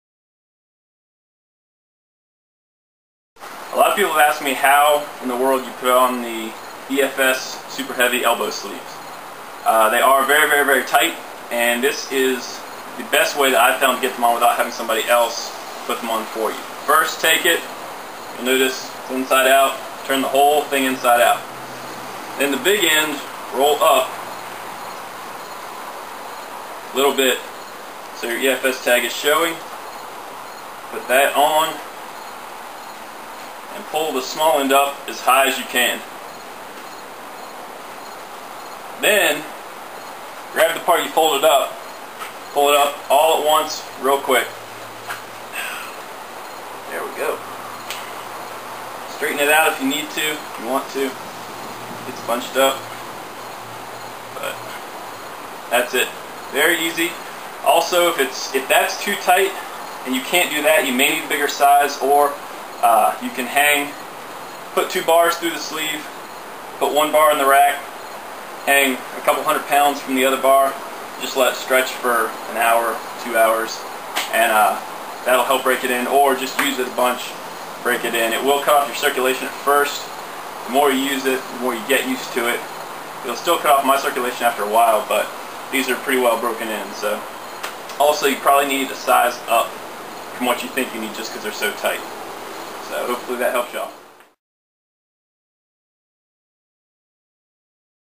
How to Put on EFS Super Heavy Elbow Sleeves
If you are gonna get the best gear out there, you ought to know how to use it too.  So here is a quick demo answer to a question that tons of people have asked me.  It is much easier to show than to explain.